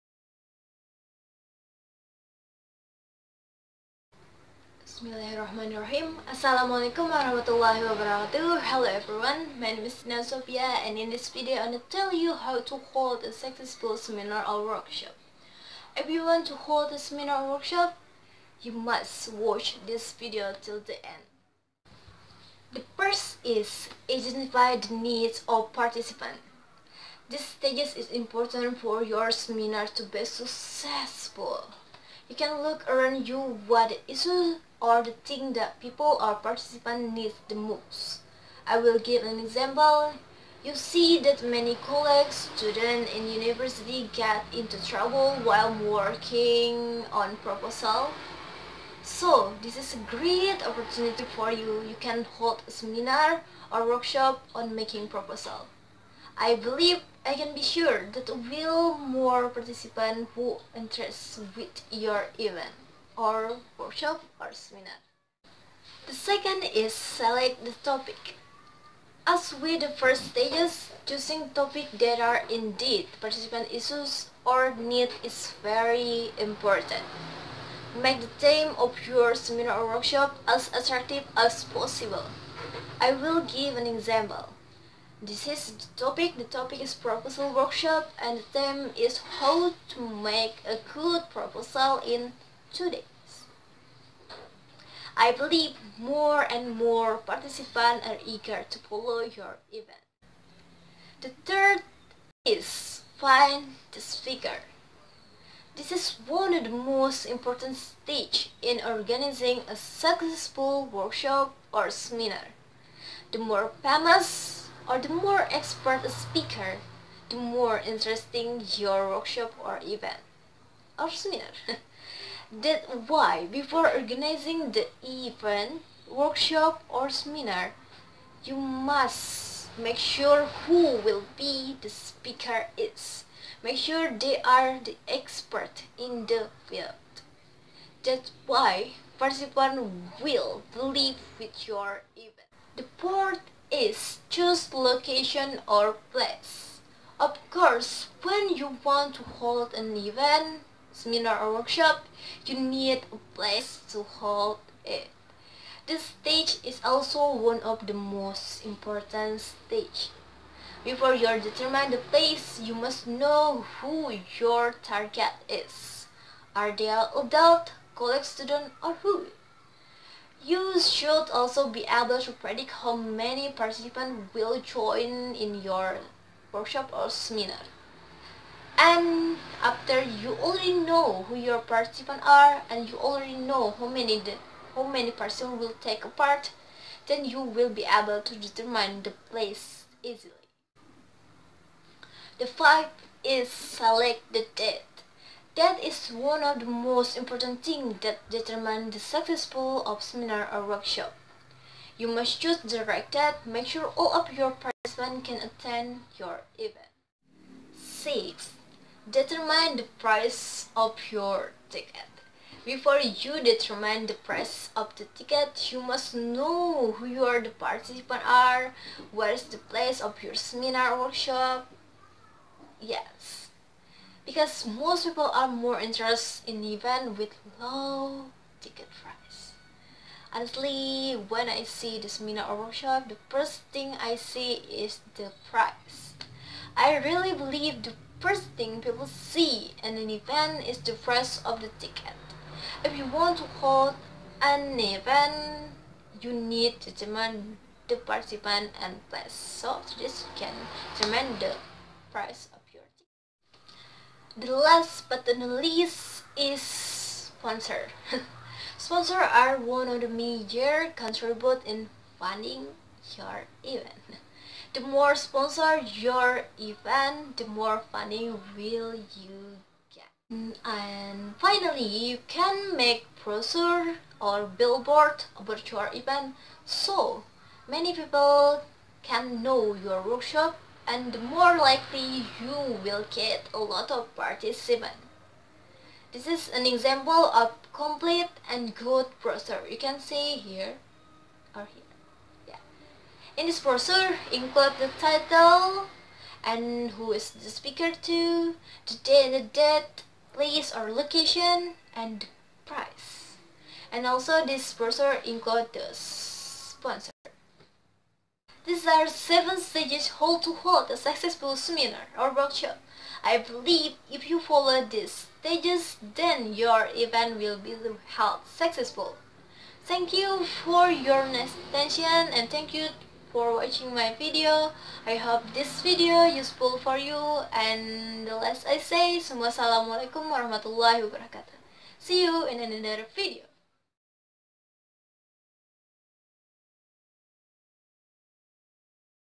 7 Stages "How to Hold a Successful Seminar or Workshop"
This is a video for my public speaking final test🎬
Thank you to Miss Afifah Linda Sari, M.Pd as a Public Speaking lecturer💕
You may give a comments for my video